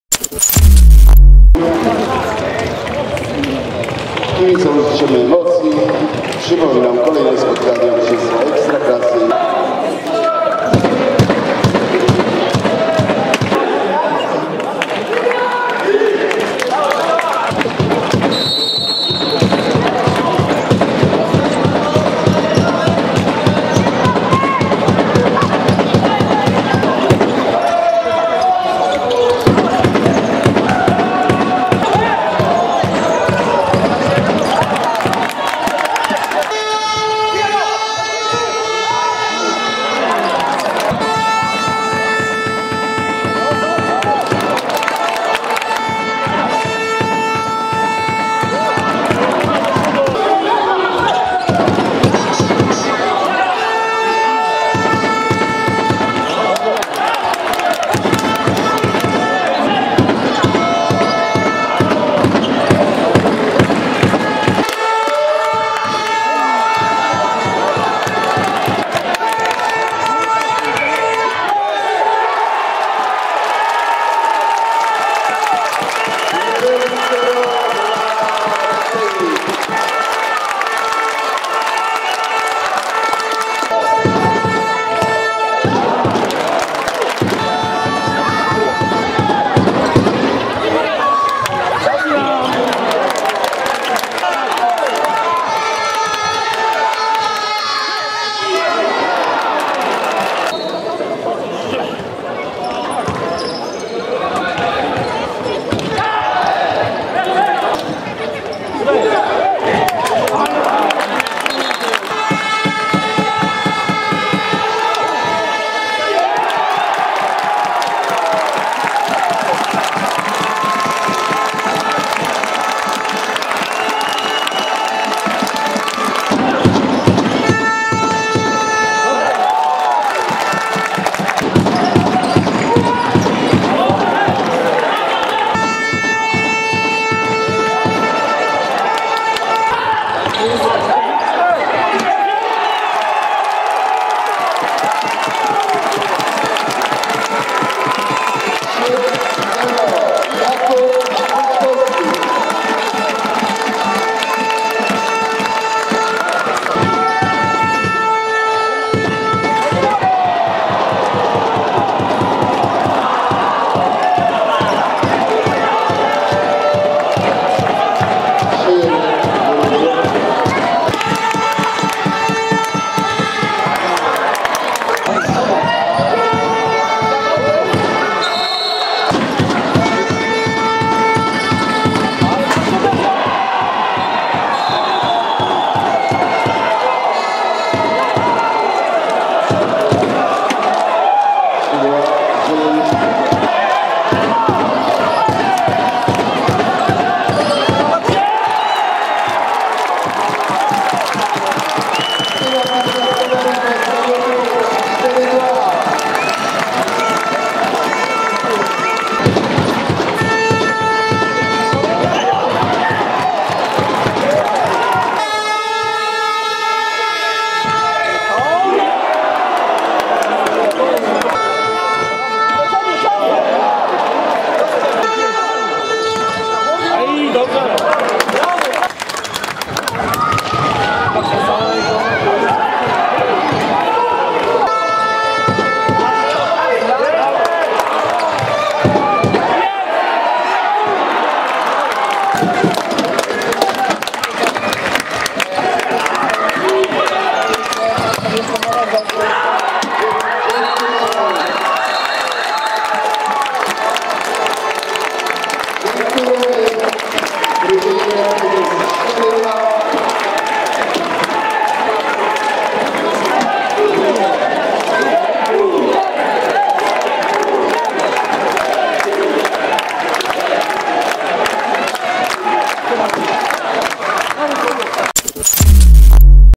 Red Devils Chojnice 4-2 Acana Orzeł Futsal Jelcz-Laskowice / Skrót / Futsal Ekstraklasa [20.10.2018]
Zobacz najciekawsze akcje meczu Futsal Ekstraklasy: Red Devils Chojnice - Acana Orzeł Futsal Jelcz-Laskowice z KP Sport